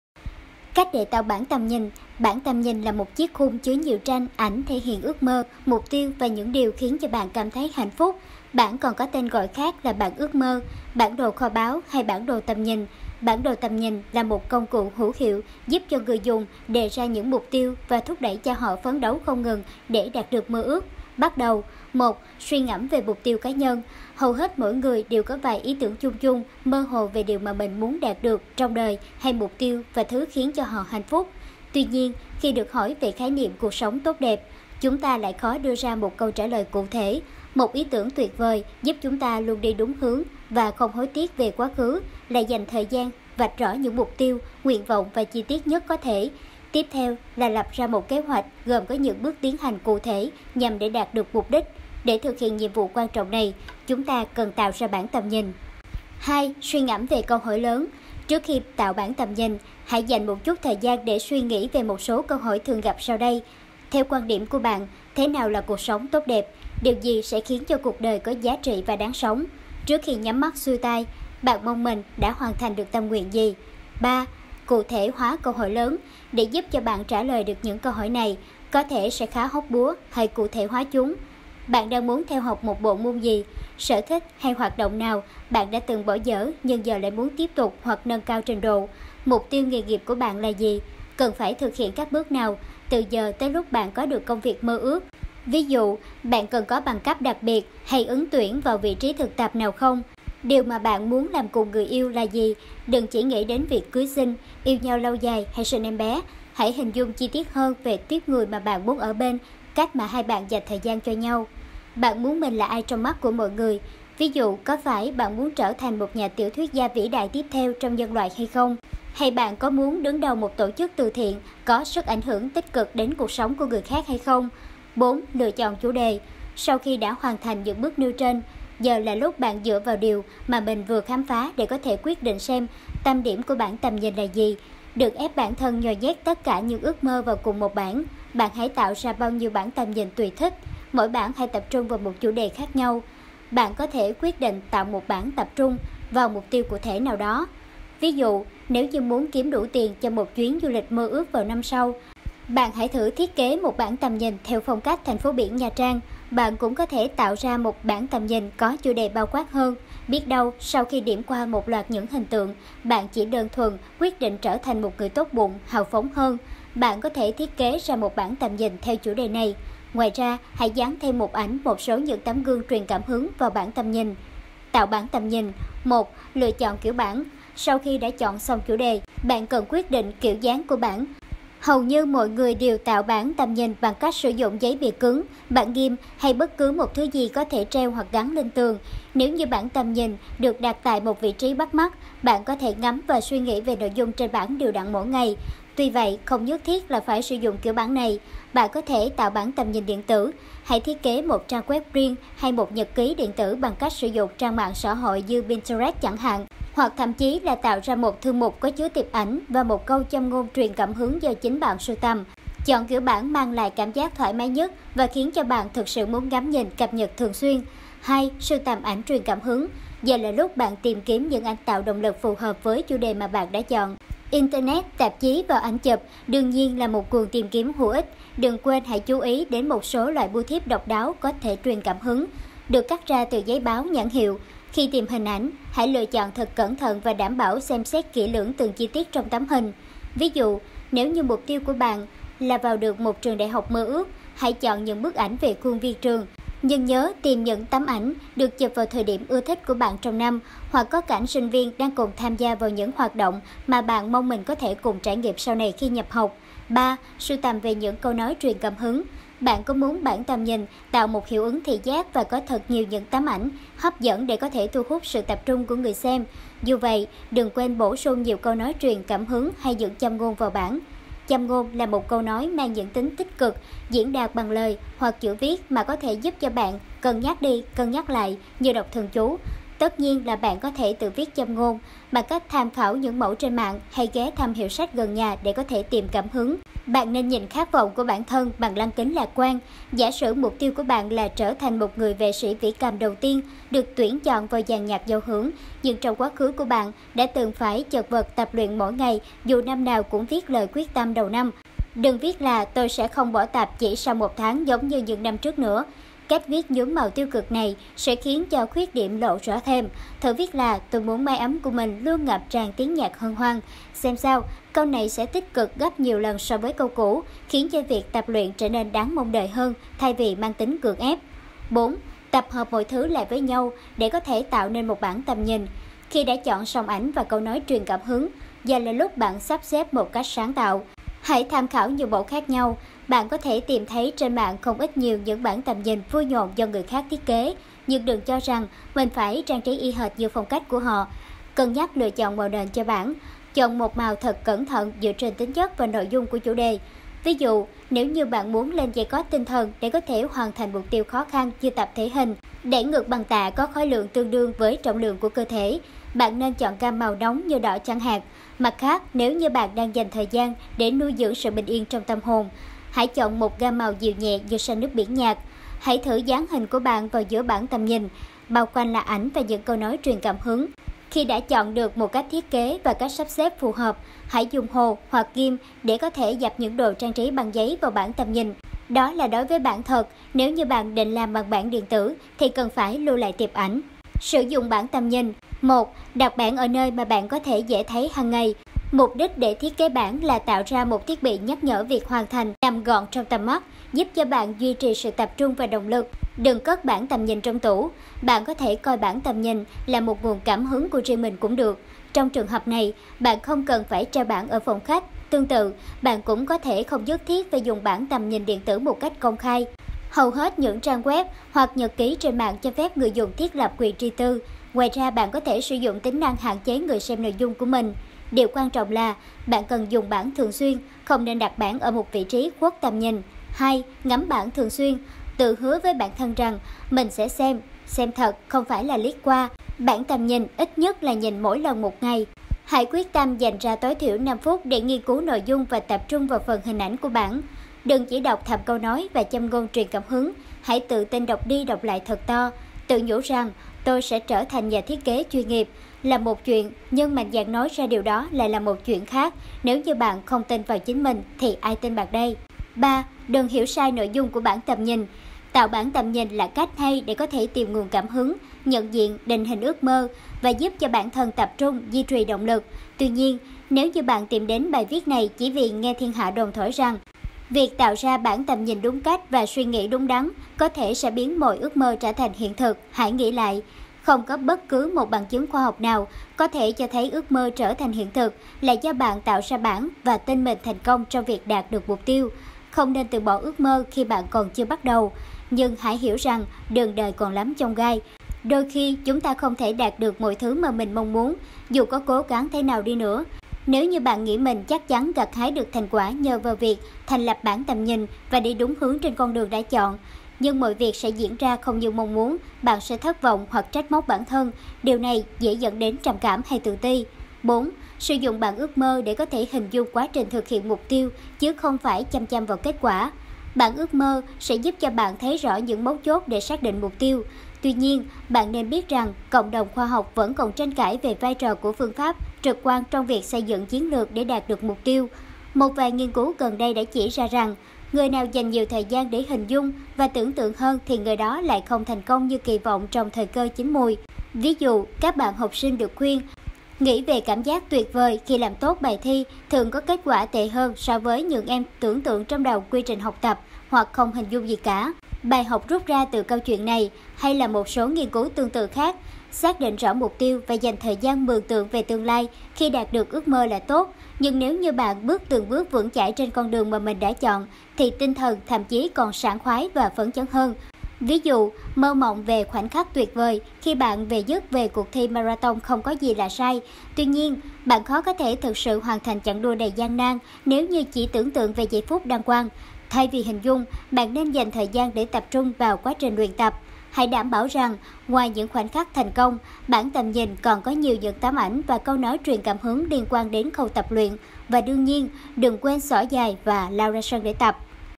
Cách để Tạo bảng tầm nhìn | WikiHow Tiếng Việt | Vietnamese
Bảng tầm nhìn là chiếc khung chứa nhiều tranh, ảnh thể hiện ước mơ, mục tiêu và những điều khiến bạn thấy hạnh phúc. Bảng còn có tên gọi khác là bảng ước mơ, bản đồ kho báu hay bản đồ tầm nhìn. Bảng tầm nhìn là công cụ hữu hiệu giúp người dùng đề ra mục tiêu và thúc đẩy họ phấn đấu không ngừng để đạt được ước mơ